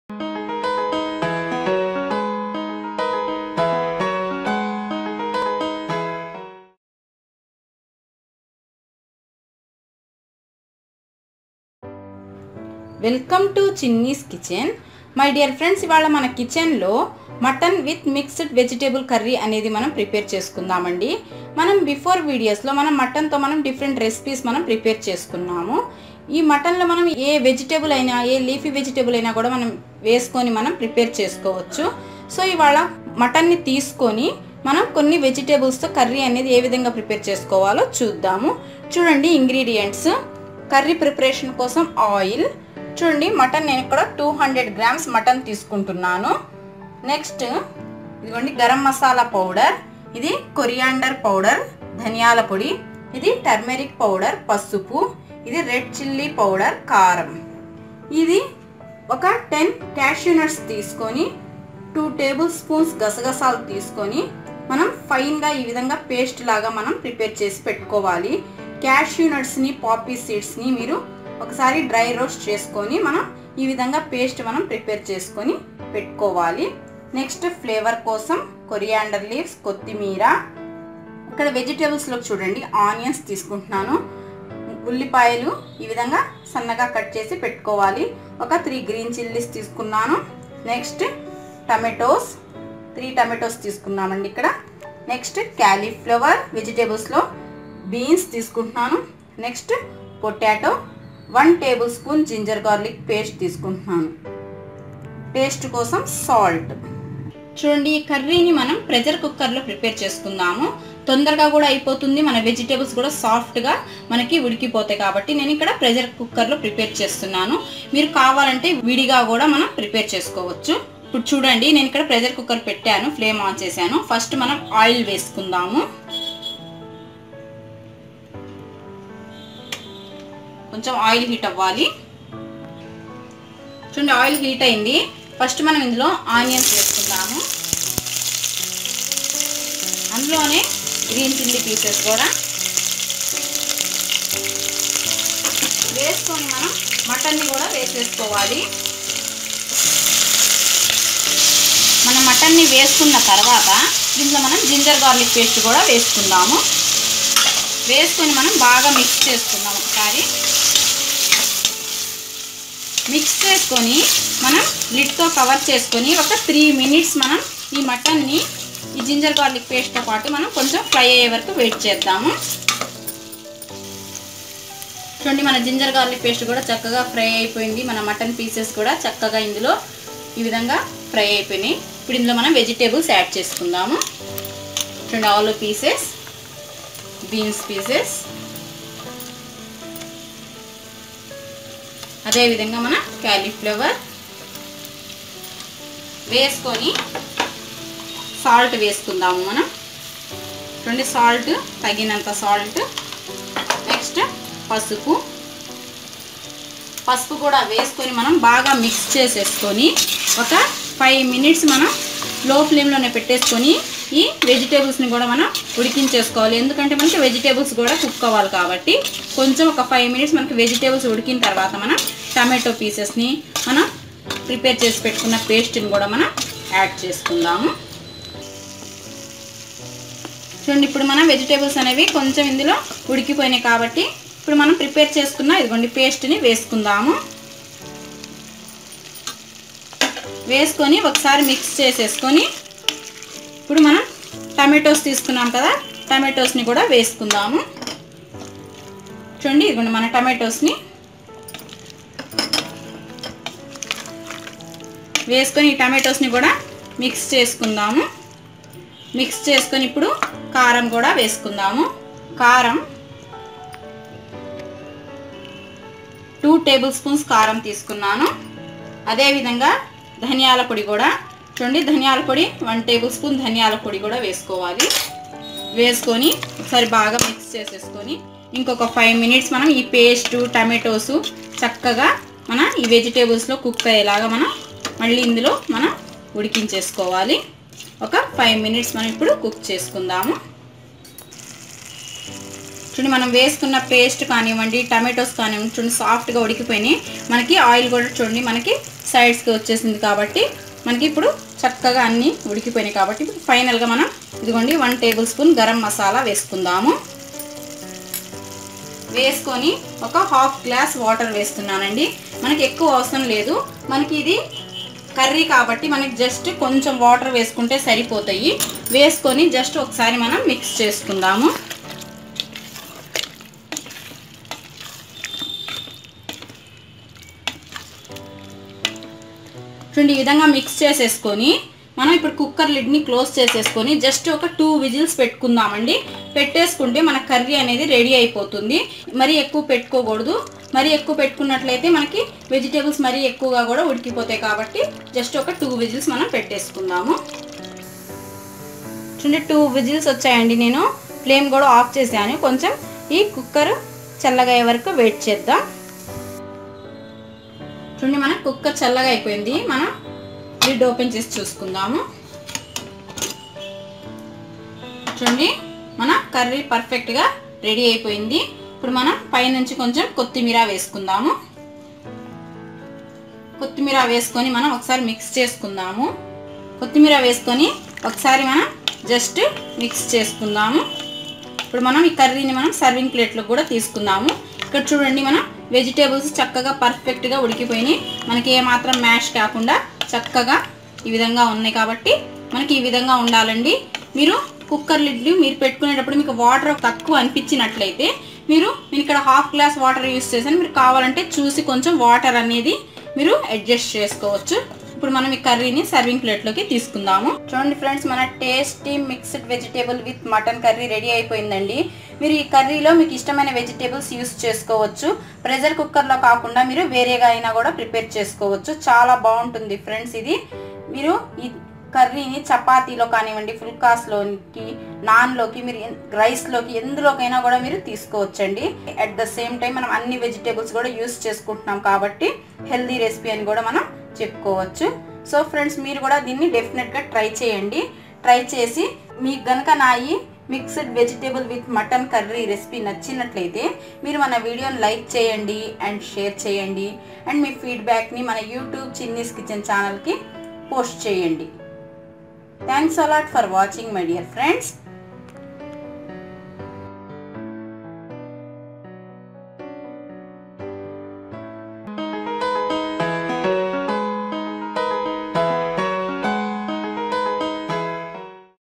MIXED VEGETABLE WITH MUTTON CURRY [పోషకాలు కలిగిన కూరగాయలు మరియు వేట మాంసము కలిపిన కూర ]
mutton-200g, vegetables-as our wish, tomatoes-3, ginger garlic paste 1-tbs,onions, green chillies-3,coriander powder1-tbs,garam masala powder-1tbs,oil, salt, red chiili powder-2tbs,10 cashew nuts with 2tbs poppy seeds paste,coriander leaves.